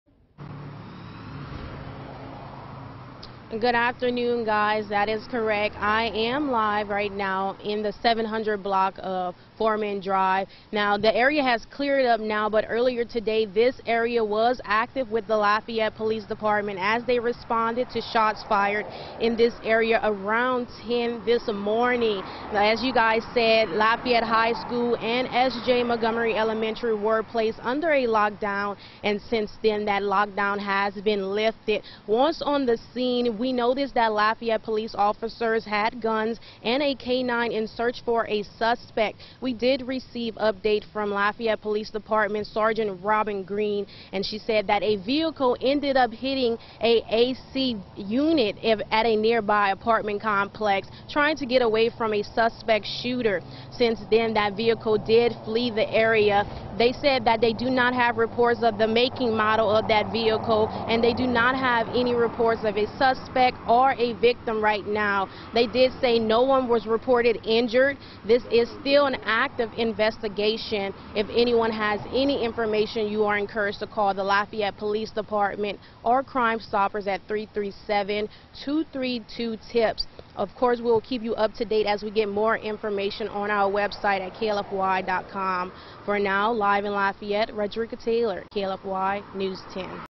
Shots fired near two Lafayette schools
Lafayette Police confirmed they are investigating a shots-fired call in the 700 block of Foreman Dr., one block away from S.J. Montgomery Elementary.